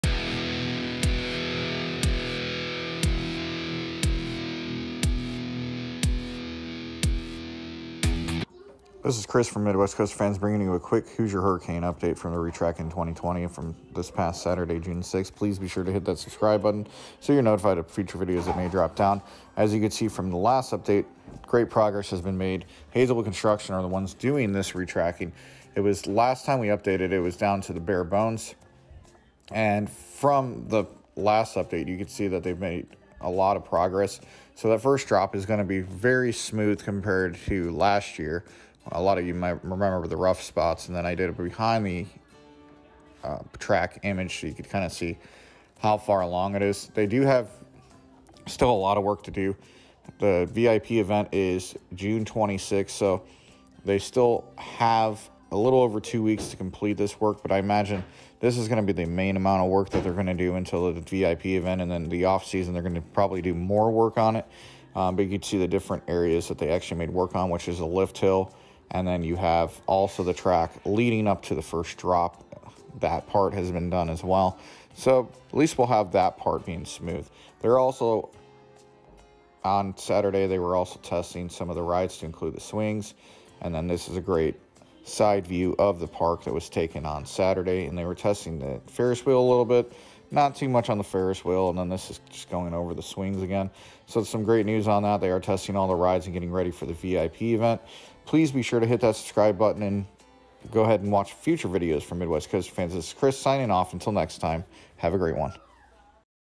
Hoosier Hurricane Re-Track Update | Indiana Beach | June 6, 2020
Hoosier Hurricane Re-Track Update. Hazelwood Construction has made great progress since our last update.